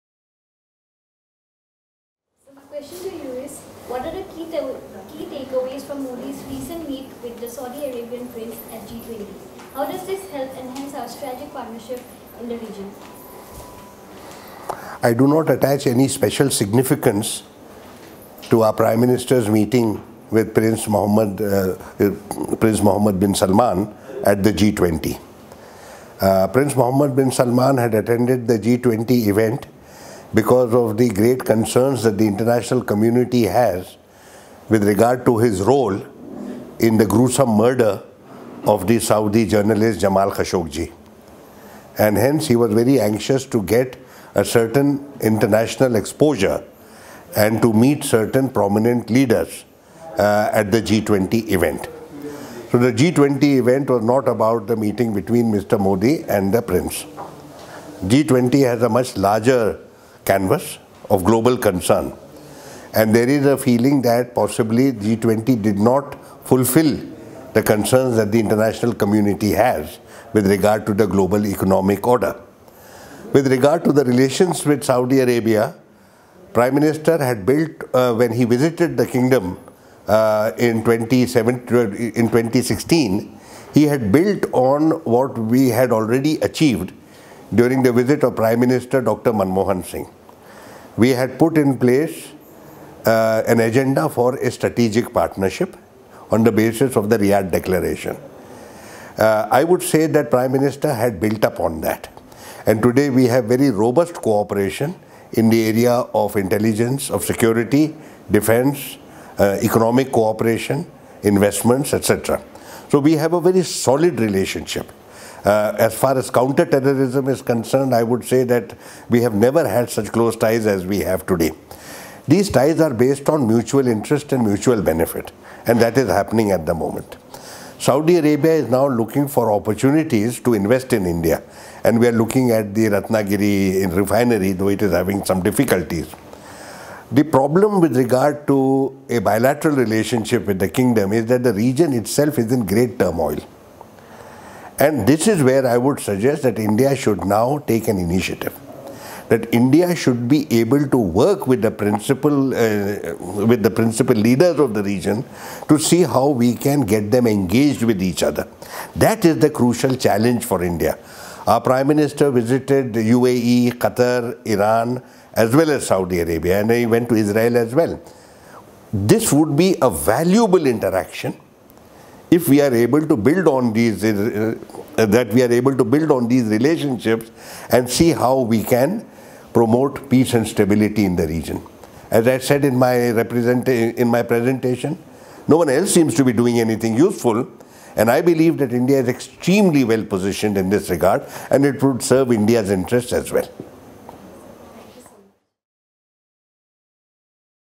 IRC 2018: Ambassador Talmiz Ahmad
Ambassador Talmiz Ahmad, Ram Sathe Chair in International Studies, Symbiosis International ( Deemed University) shared his views on India's relations with West Asia